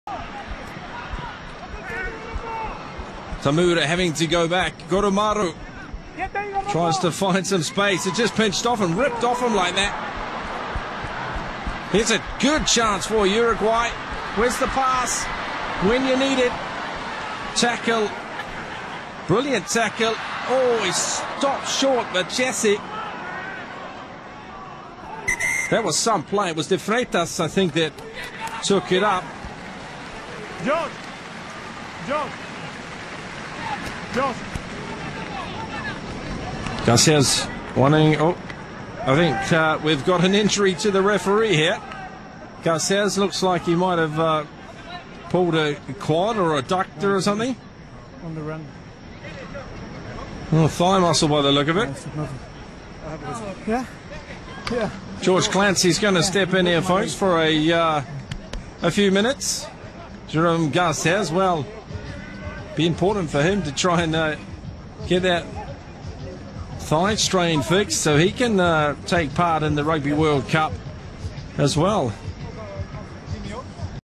Jérôme Garcès World Cup put in doubt with abductor injury
Referee Jérôme Garcès is forced off the pitch with an injury to his abductor muscle during the Rugby World Cup warm up match between Japan and Uruguay at the Level 5 Stadium in Fukuoka in August 2015.